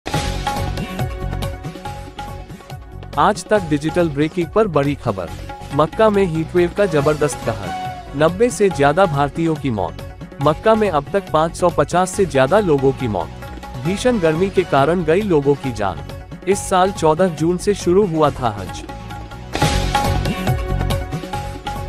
Breaking News: मक्का में Heat Wave के बीच हज यात्रा के दौरान अबतक 90 भारतीयों ने गंवाई जान
सऊदी अरब के मक्का में गर्मी का सितम थमता नहीं दिख रहा है. भीषण गर्मी और हीटवेन के बीच अब तक यहां 90 भारतीयों सहित अलग-अलग देशों के 550 से ज्यादा लोगों की मौत हो चुकी है. इन मौतों कारण हीटवेव या इलनेस (कोई बीमारी) बताया जा रहा है. सभी लोगों की मौत नेचुरली हुई है. यानी इनमें से कोई भी हादसे का शिकार नहीं हुआ है और ना ही किसी के भी शरीर पर चोट के कोई निशान पाए गए हैं.